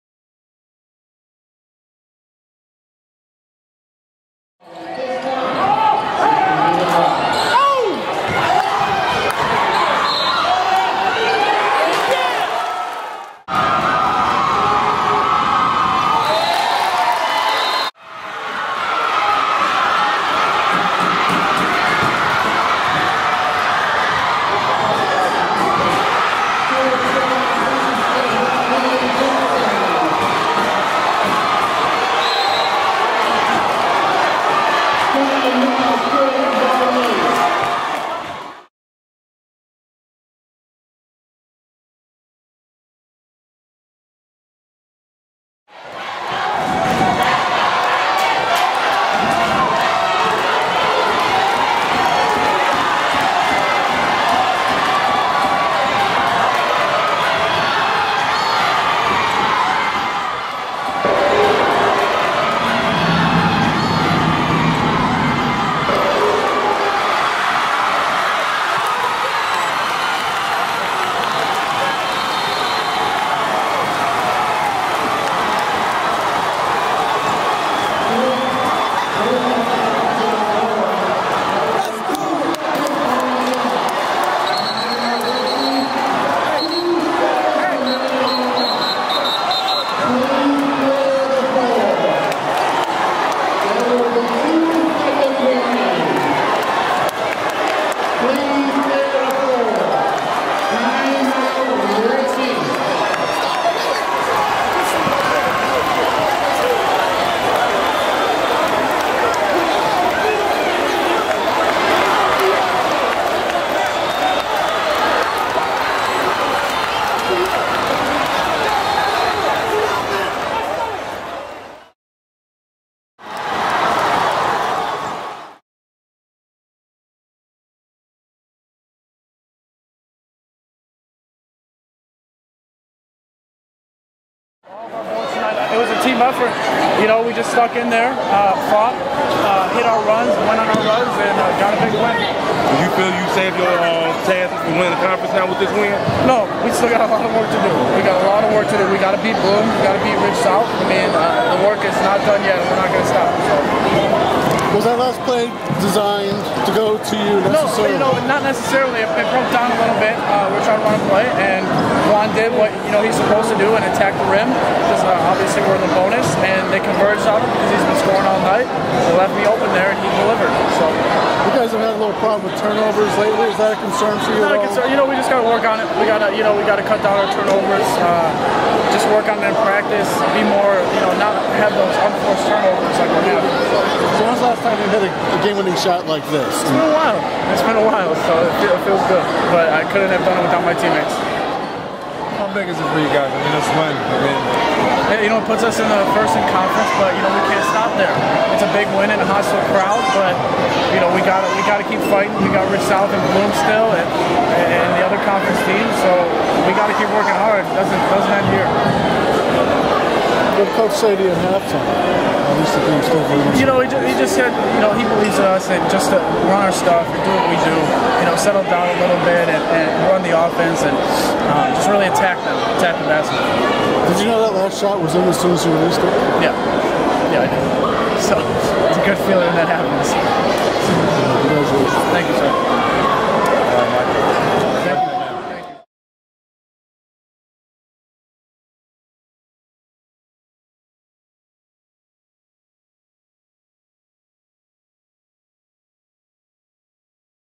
Michael Orris Hits Game Winning Shot Class 2012 Illinois Recruit Bonus Interview
It was a team effort and Michael Orris showed everyone why he will be in the Big Ten this fall watch the interview at the end...